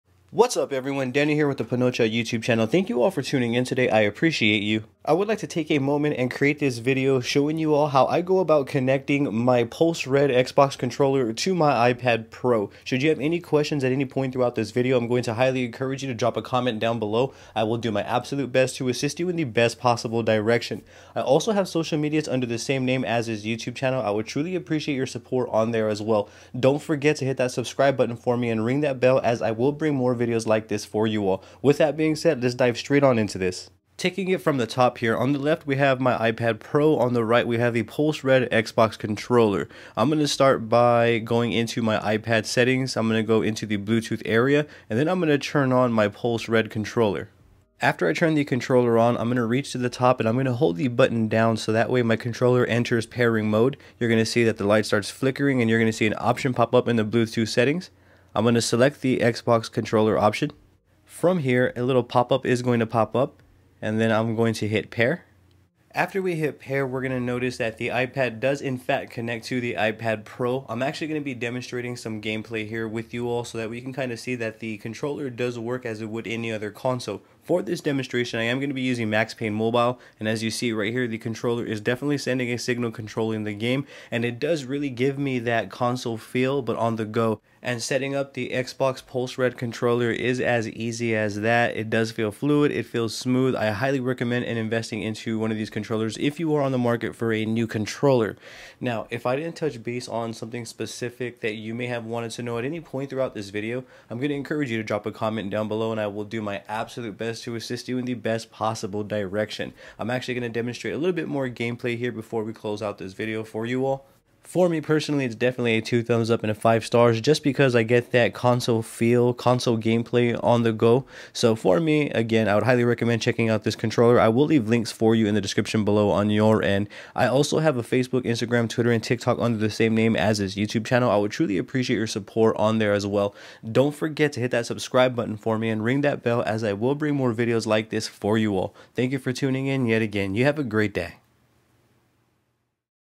Connecting Xbox Controller To iPad Pro
Are you on the market for a new Xbox controller?

In this video we will connect the Red Pulse Xbox controller with my iPad Pro. The connection process is super simple and by the end of the video you will know how to connect the controller with your tablet or iPad on your end.

I personally love this controller and I highly recommend it to anyone whom is looking for a controller! Not only can we use this controller on our xbox series consoles we can also use this controller on windows, android, iOS, making this controller a  must have for any gamer!

I will link you below so you can check out this Red Pulse Controller on your end.

Pulse Red Xbox Controller

Intro 0:00
Where To Begin 0:33
iPad Gameplay Testing 1:10
Outro 2:00